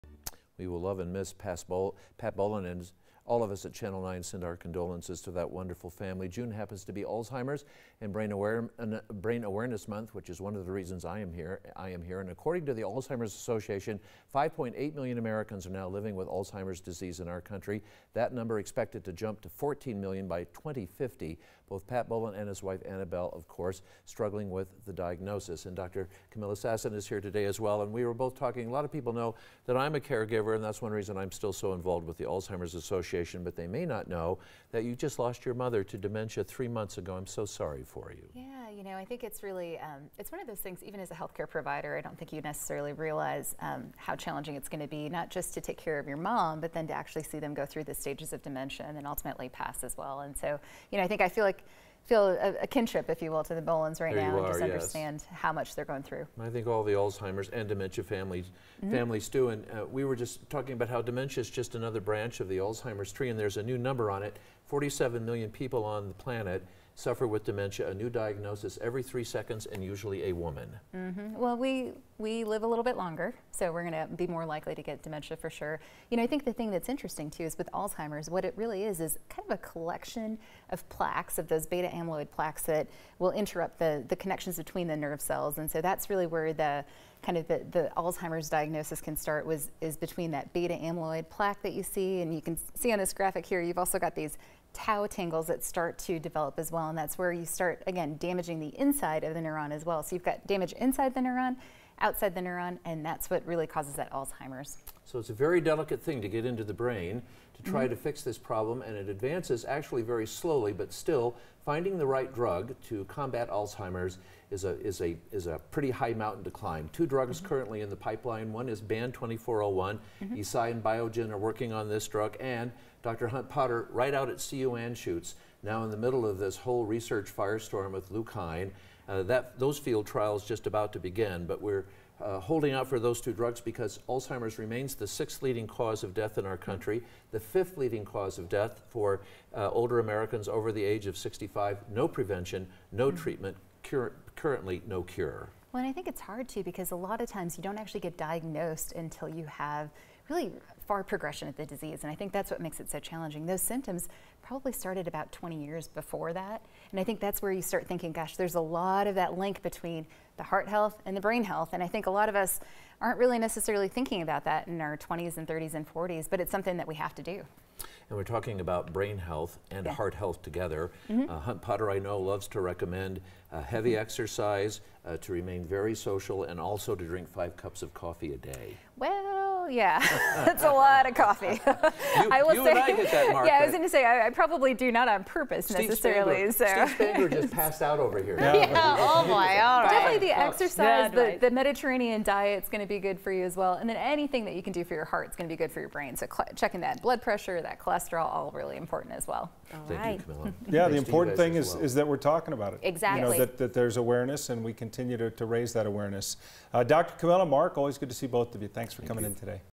Alzheimer's disease and how it progresses
According to the Alzheimer’s Association, 5.8 million Americans are living with Alzheimer’s. This number is projected to skyrocket to 14 million people by 2050.

9NEWS (KUSA) is located in Denver, Colorado.